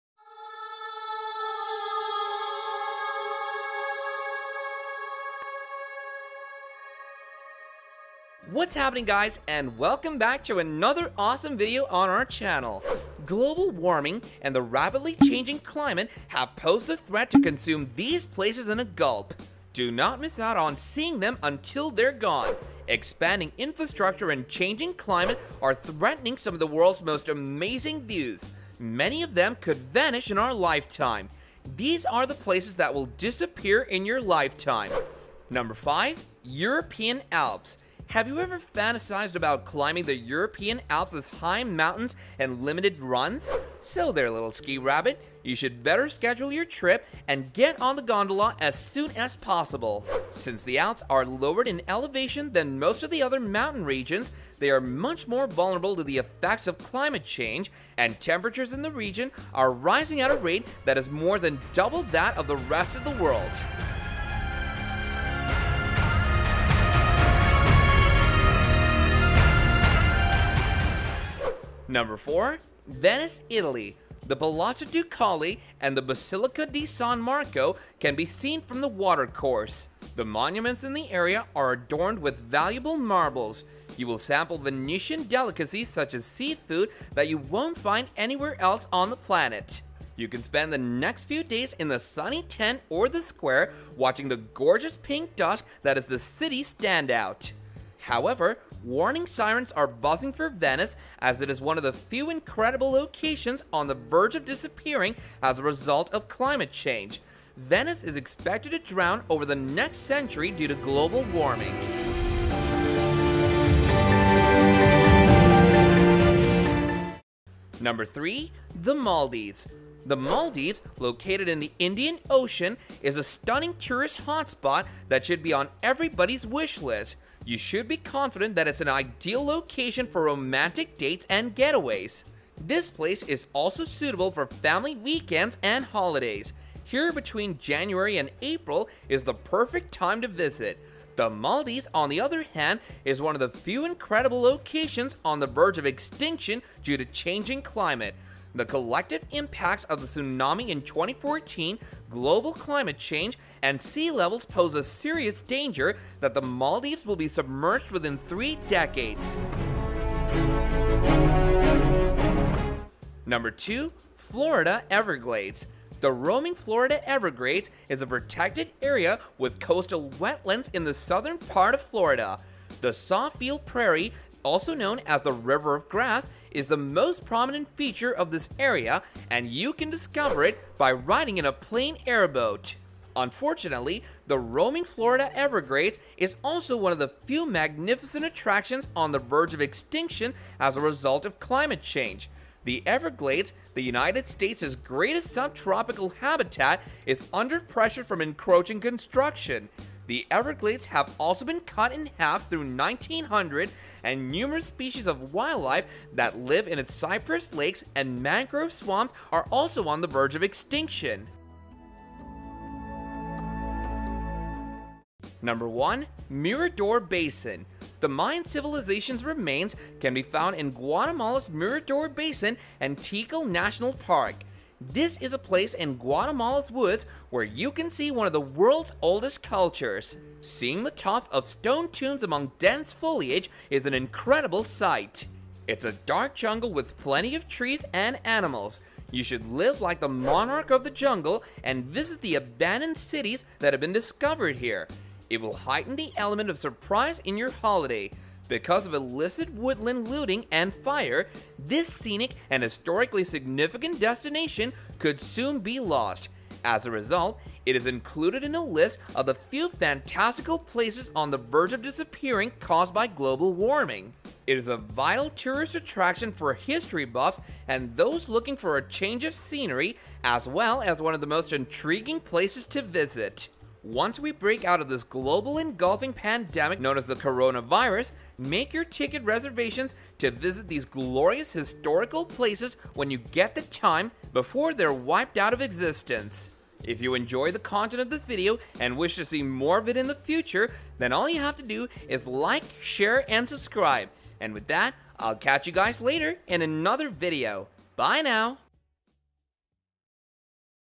Places That Will DISAPPEAR In Your Lifetime
Join us in today’s video as we show you the places that will disappear in your lifetime. While dangers to the world’s most beloved destinations used to merely include erosion and climate change, there are innumerable factors that play a part in the destruction of a historical or natural site. With the looming extinction of several of Earth’s species and ecosystems, the focus of tourism is shifting. Tourists are now rushing to be the ‘last’ to encounter and experience a place or a species.

There are destinations in this beautiful world that are disappearing right in front of our eyes. So, before you miss one more thing, consider making your own bucket list. Live in the moment and don’t miss these wonderful places that will disappear soon.